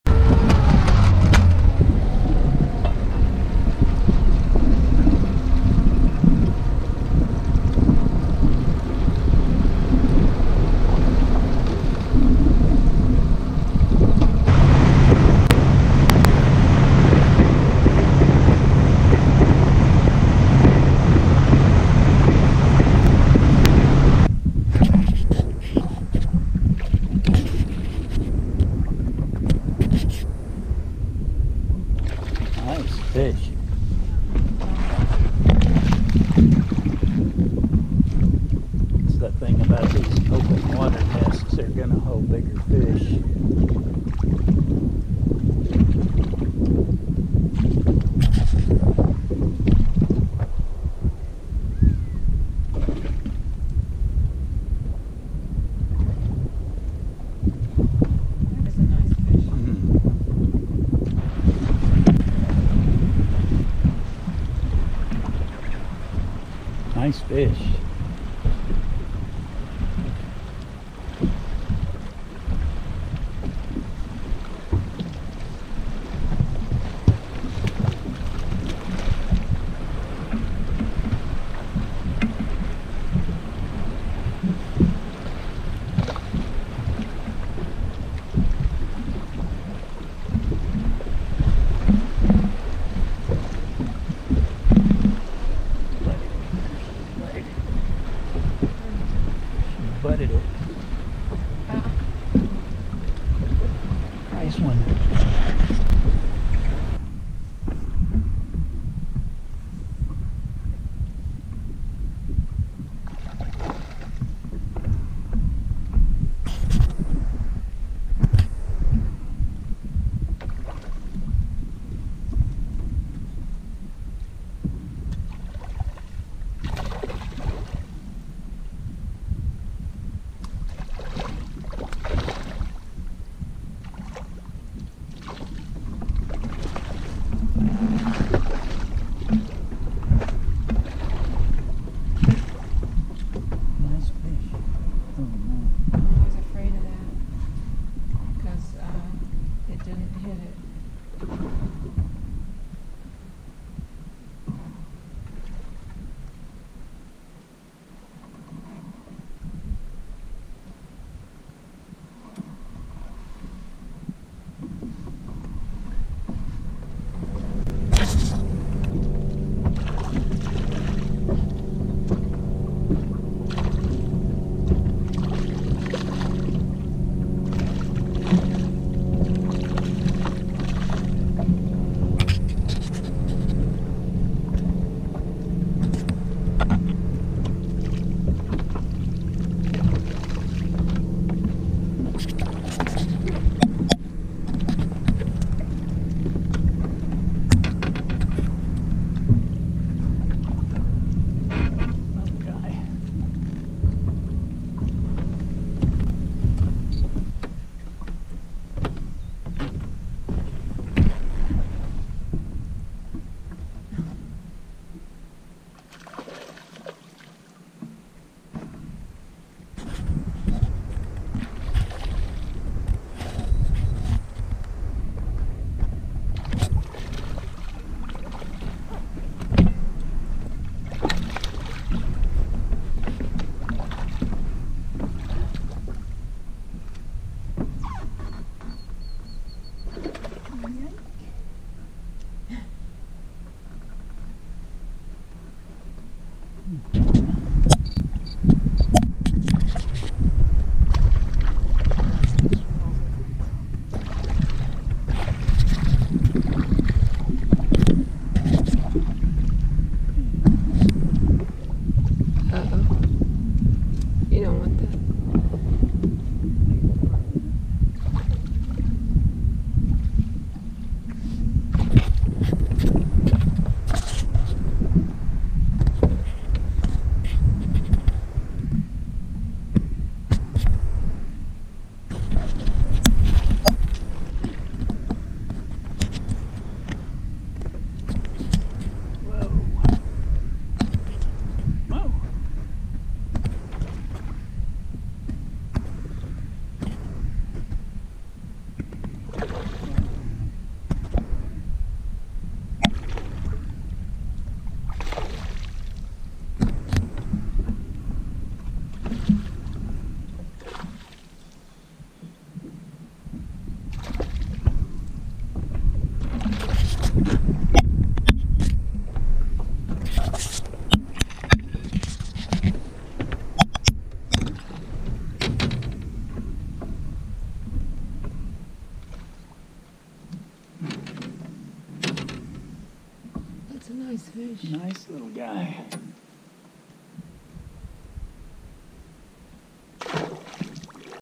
Tired of the Ski Slopes for the Season? Try Arizona Bass Fishing!
Take a Spring Break vacation to Lake Mohave Arizona. Spring bass fishing is outstanding in crystal clear water.  Spring is an ideal time for boating on Lake Mohave.  Summer is the high season!